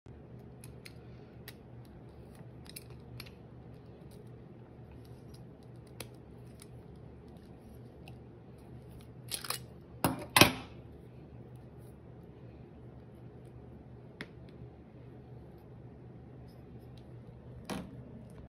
Squeeze Oil Paint Tubes and Get Every Drop
You want to get every last drop out of those difficult-to-squeeze metal oil paint tubes. Make it easy on yourself by using our heavy-duty TubeWringer!

Metal tubes are easy to squeeze with our American-made TubeWringer.